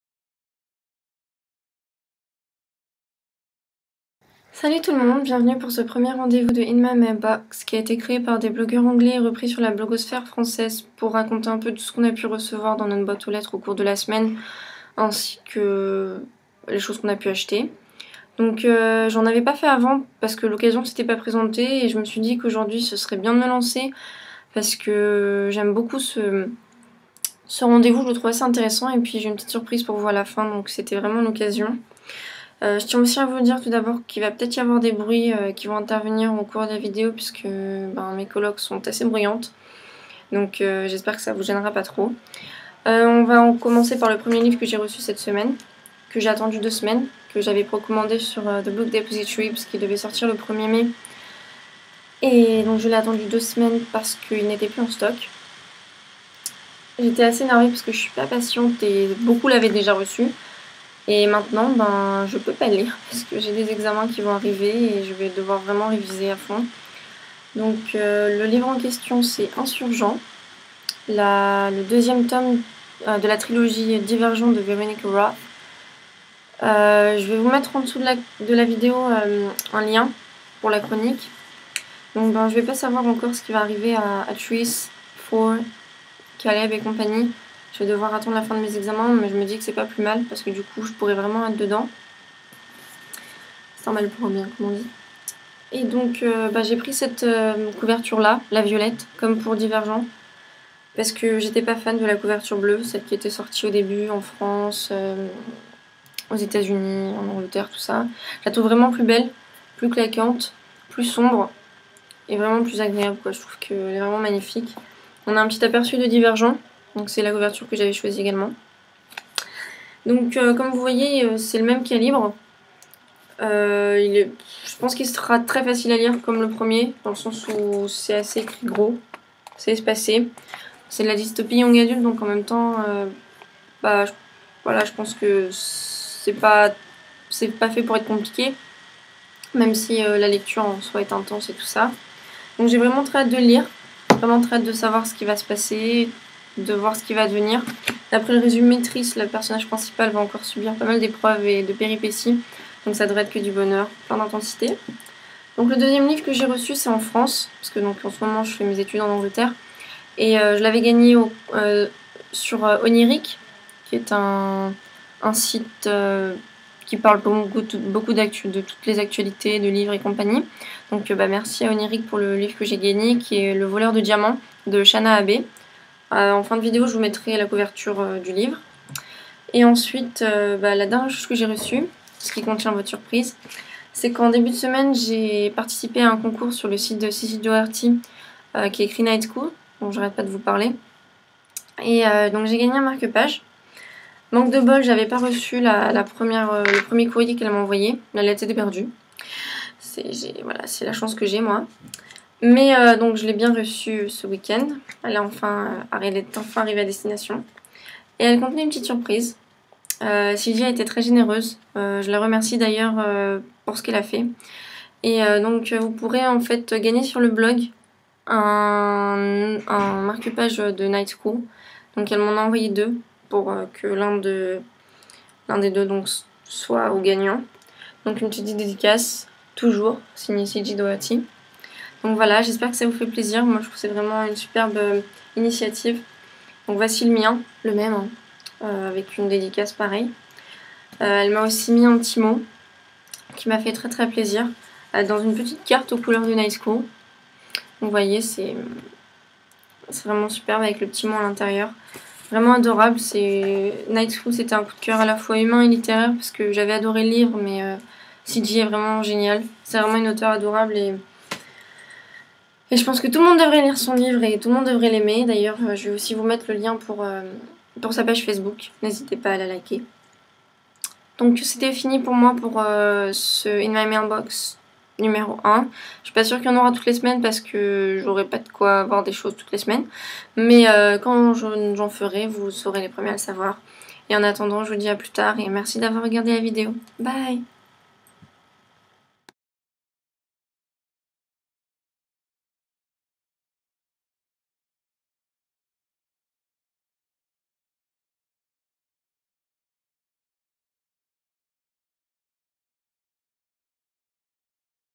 In My Mailbox #1
In My Mailbox : Rendez-vous hebdomadaire créé sur des sites anglais et repris sur la blogosphère française.

Et parce que j'ai oublié de mettre la couverture du Voleur de Diamants de Shana Abé

Encore une fois, désolée pour le son et le bruit de l'ordi qui chauffe. Il va falloir que je vois comment régler ça. Dès que je lance Photo Booth, mon mac part en vrille...
Et désolée pour les cafouillages à des moments, je finirai par maîtriser l'art de la vidéo, promis ;p